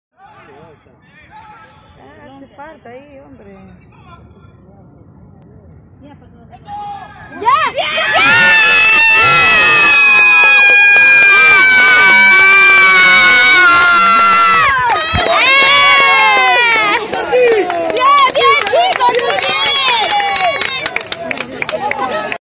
Apoyando a mi equipo
Regresando de una lesion despues de mucho y que mas que apoyando a mi equipo en la victoria de locales .